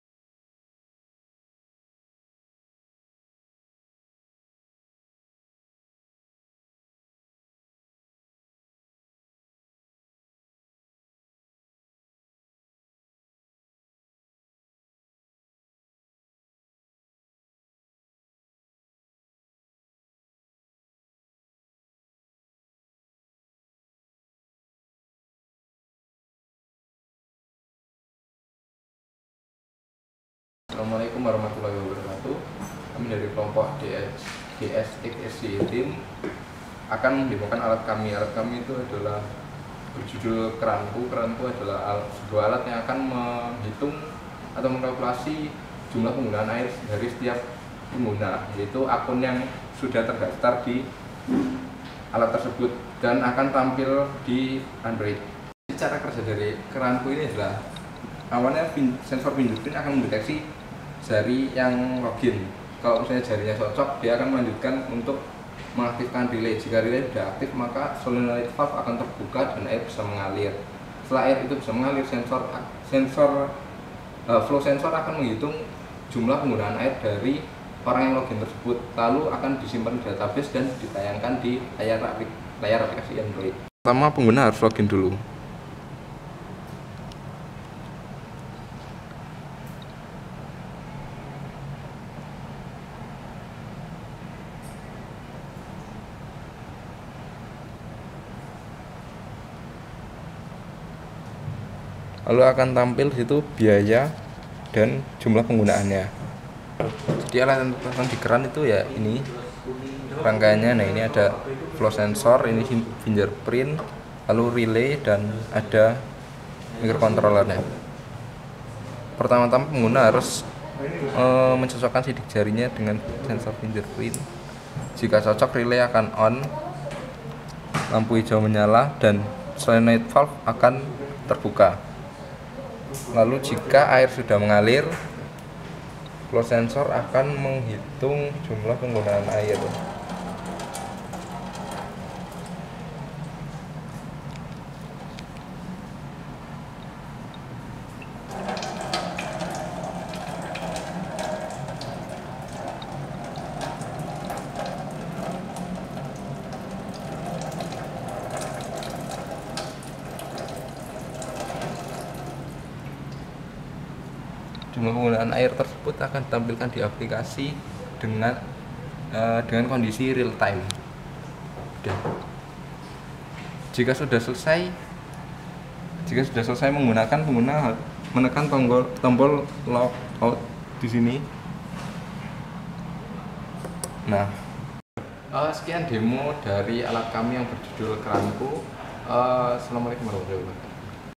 DSXSDE TEAM - kranku.
kranku. adalah alat yang mampu menghitung penggunaan air dari setiap user yang telah terdaftar dan data dikirim ke aplikasi android. Hasil perhitungan jumlah penggunaan air akan dikalkulasikan sehingga biaya air akan diketahui. Biaya dan jumlah air yang telah digunakan akan ditayangkan di android setelah user login di aplikasi.